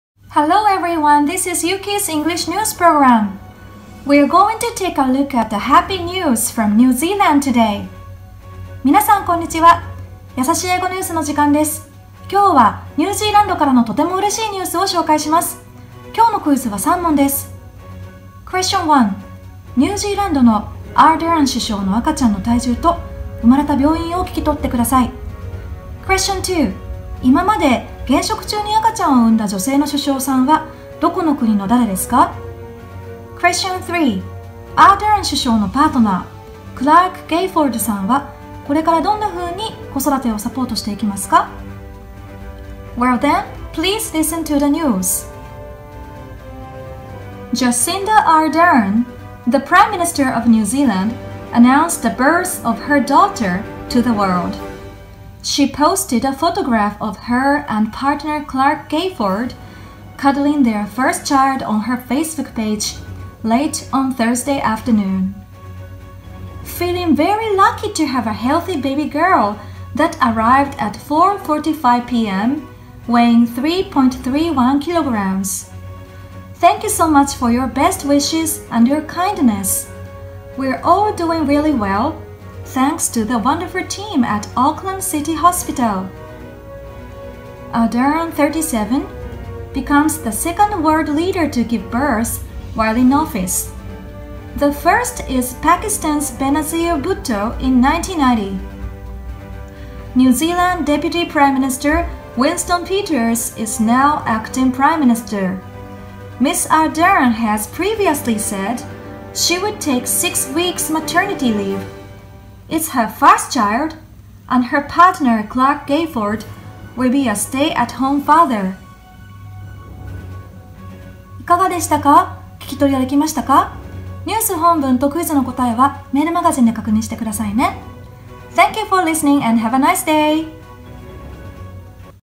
やさしい英語ニュース★Vol.180 ニュージーランドの首相、赤ちゃんを出産
中学生くらいの英語でニュースを読んでみよう！
ラジオニュースを聞いて、クイズに挑戦してね♫
答えと本文はメールマガジンで！